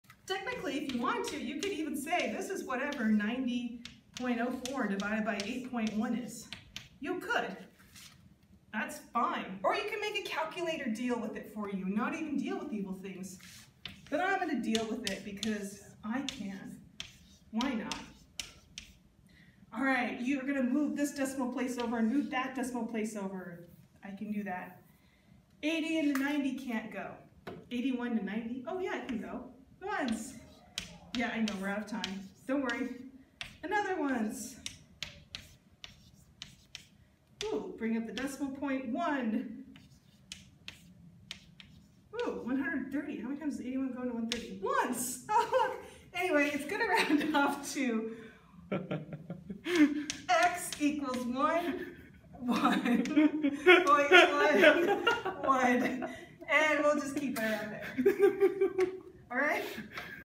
Sarah will brighten your day with math (part 3)
Sarah does division without a calculator!  Sarah will brighten your day with math (part 3)
math, but FAST!